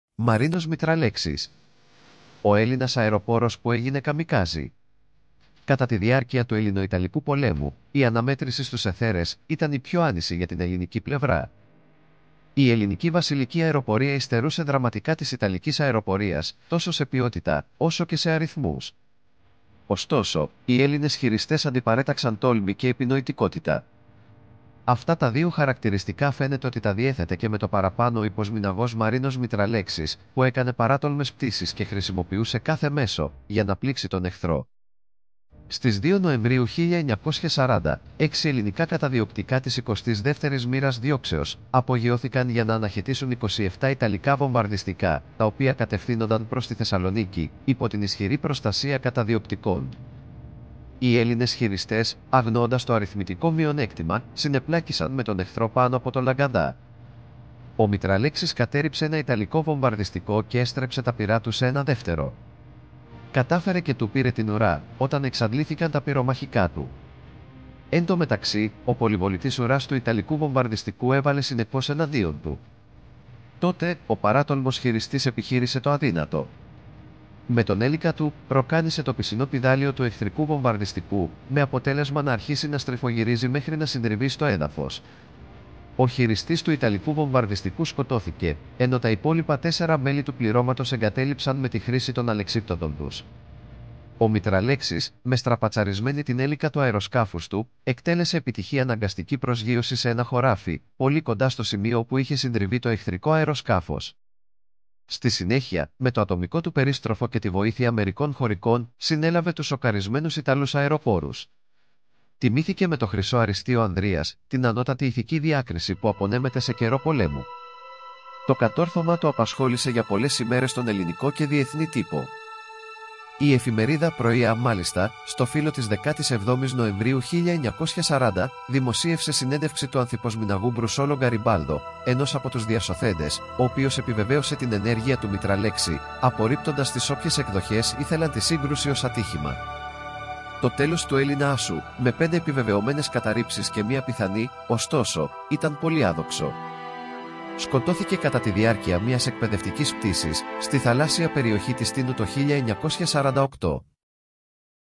Μαρίνος Μητραλέξης: O Έλληνας … καμικάζι των αιθέρων!
Ο Έλληνας που κατέρριψε αεροπλάνο χωρίς να διαθέτει πυρομαχικά